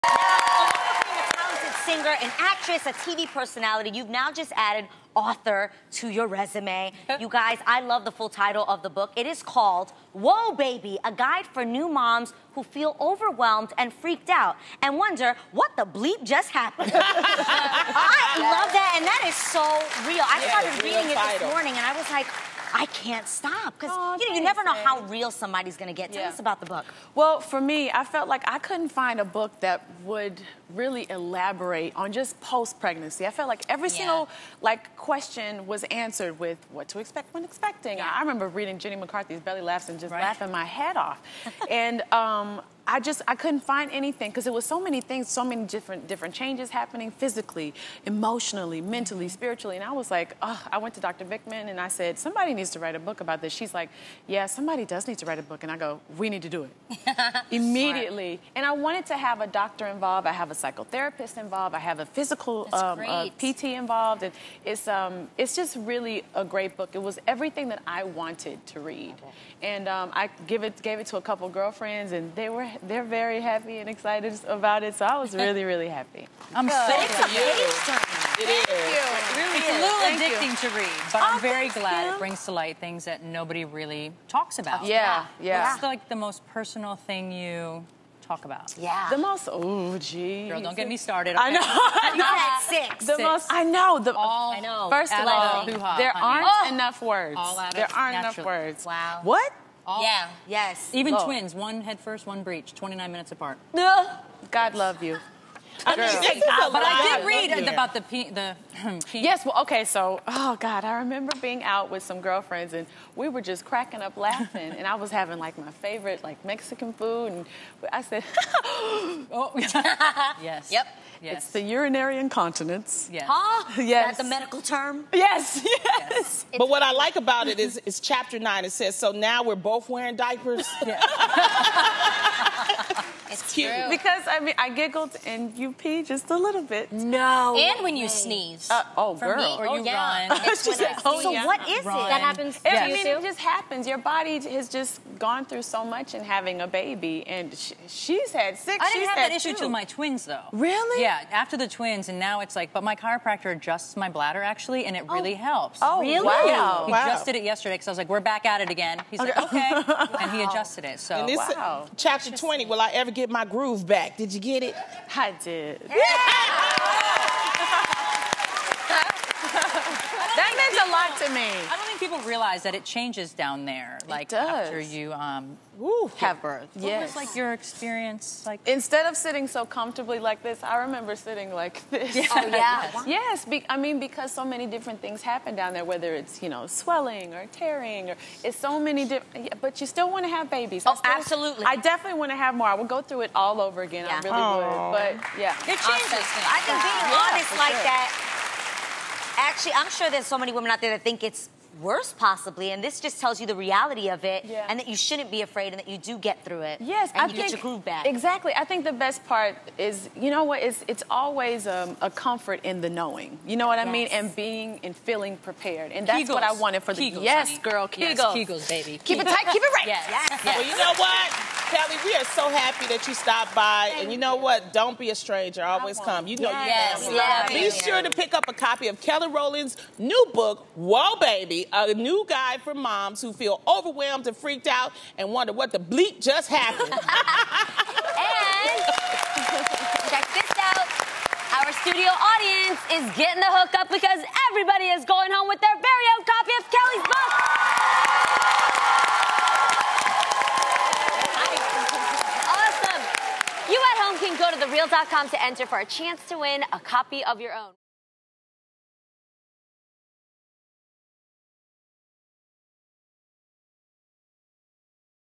How Motherhood Inspired Kelly Rowland's New Book
Kelly Rowland shares how becoming a mommy inspired her new book, "Whoa, Baby!"